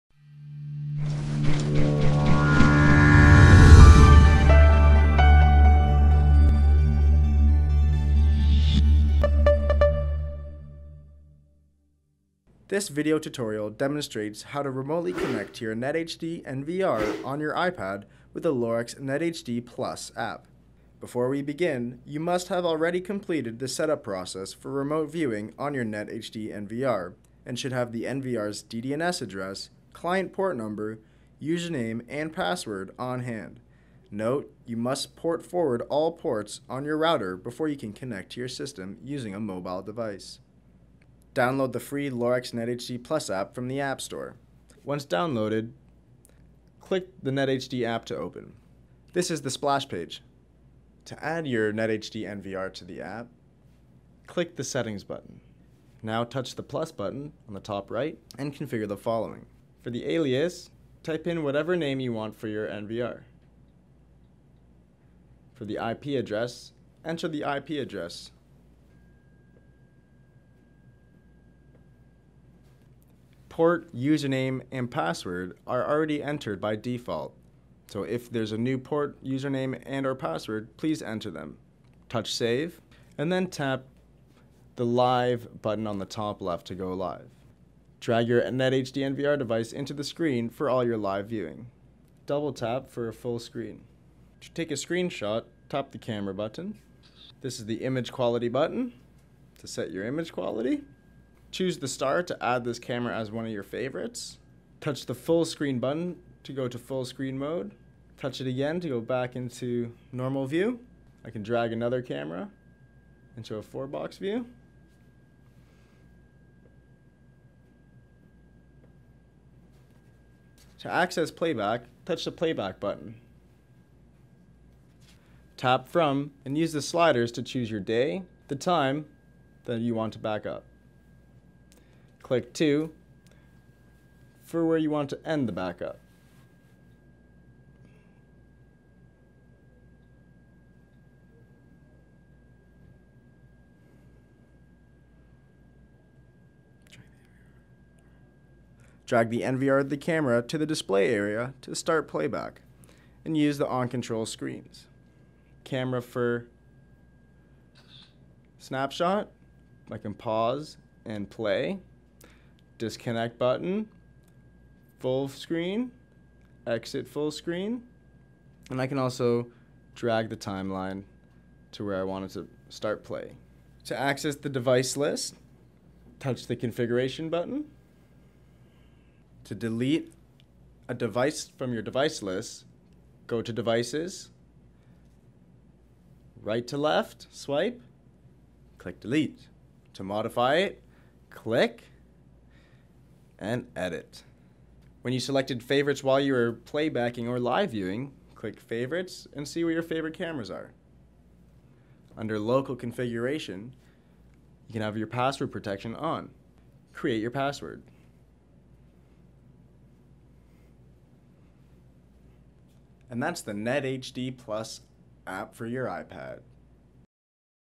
How To Setup Lorex LNR200 series netHD NVR on iPad App
View your world like never before in Full High Definition 1080p viewing and real time recording, adding a whole new dimension to your monitoring. The netHD NVR security system (LNR200) brings a more heightened level of detail and clarity to your surveillance and awareness needs.